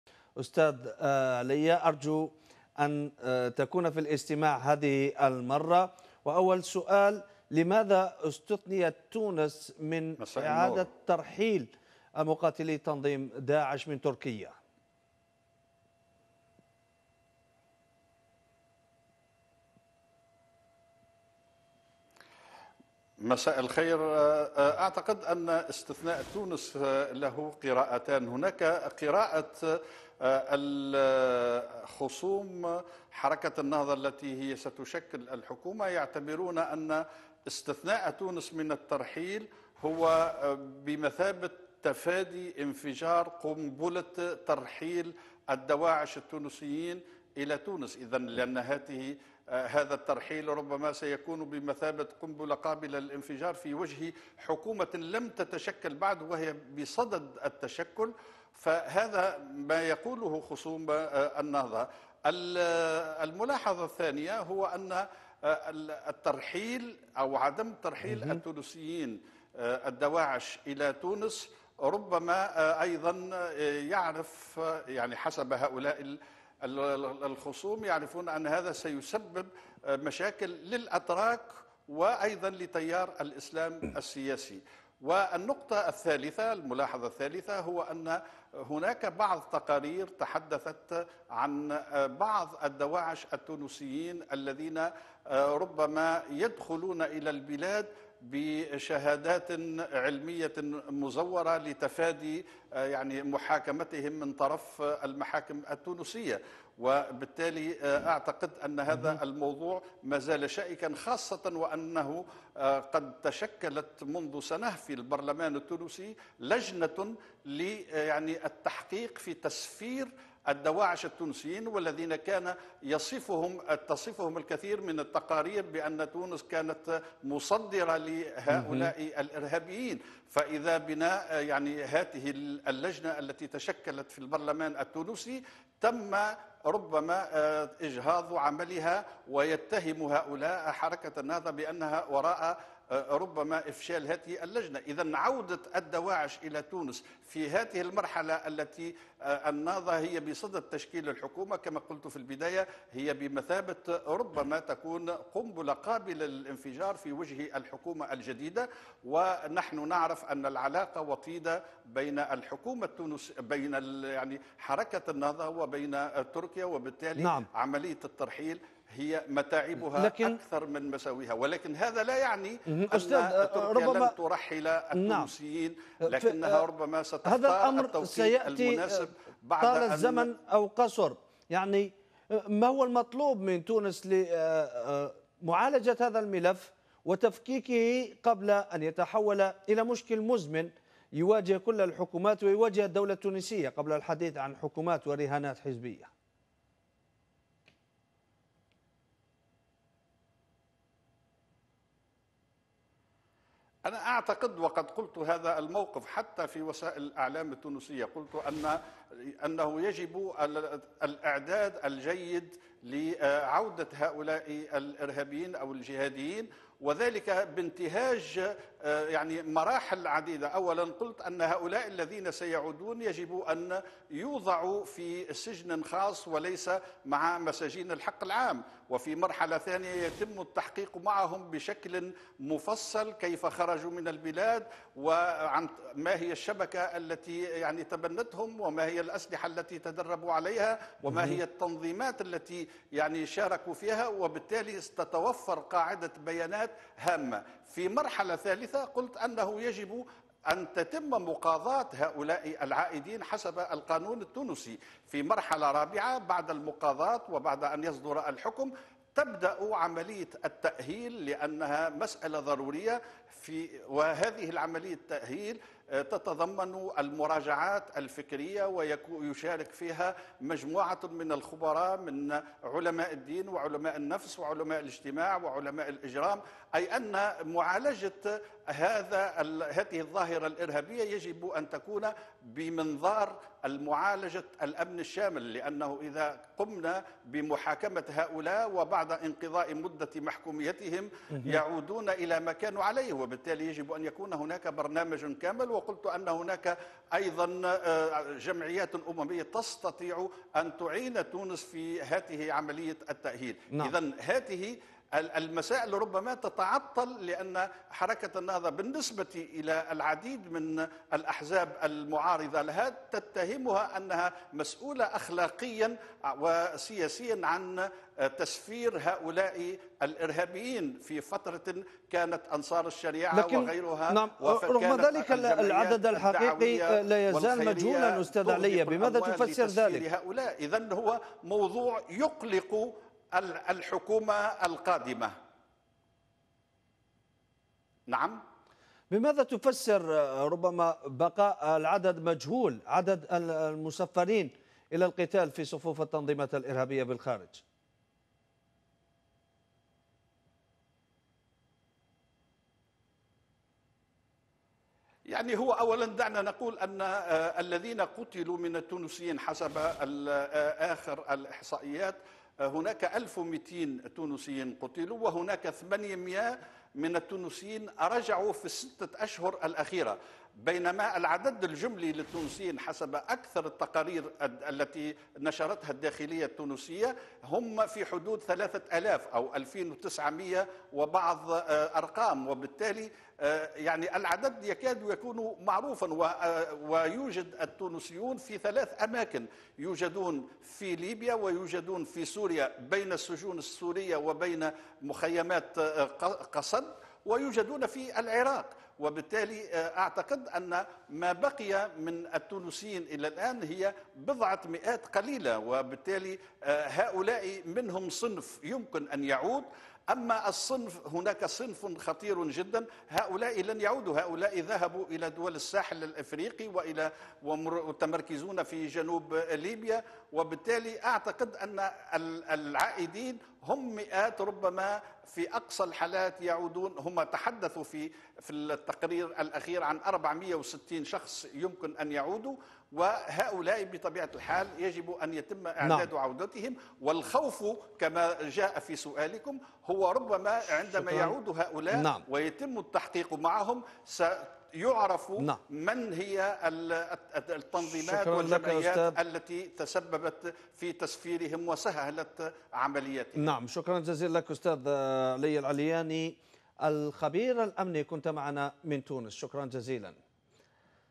حديث مع الخبير الأمني علية العلاني حول ترحيل متشددين تونسيين من تركيا إلى تونس
ترحيل متشددين تونسيين من تركيا إلى تونس